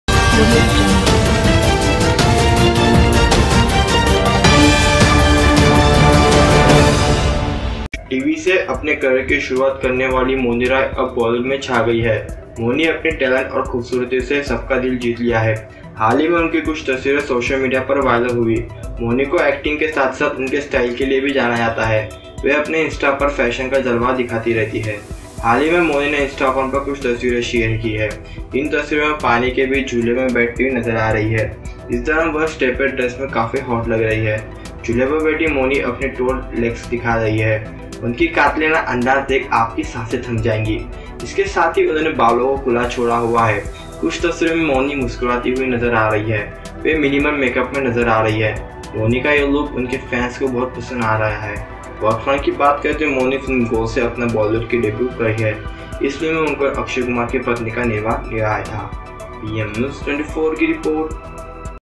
Mouni Roy Hot And Stunning Look
The hot avatar shown in Strapless Dresses, showing his beautiful legs, sitting on the swing, Mauni-roy-looks-stunning-in-this-pictures

MUMBAI: Mouni Roy, who started his career with television, is now in Bollywood. Mauni your talent and beautifully everyone's heart
 Wins. Recently some of his photographs are getting viral on social media. For the style of silk acting as well as for her style
Is known. She keeps showing the flame of fashion on her Instagram.
Recently Mauni shared some photos on the Instagram. In these photographs they are sitting on the swing between the water. During that time
Strapless dresses are looking hot enough. Mounani sitting on the hammock is showing her toned lags.
Seeing their slaughtering predictions, your sensation will stop. With this he left the hair open and swept the side. Some photos
In silence, while smiling, we are pose in some. They are looking at Minimal make-up. This look of silence loves their fans.
Talking about workfront, Mauni started her Bollywood career with Film Gold. Akshay Kumar's wife in this film
Had played the role. After this, his second film, Romeo Akbar Walter, was released, in which his pair was made with John Abraham.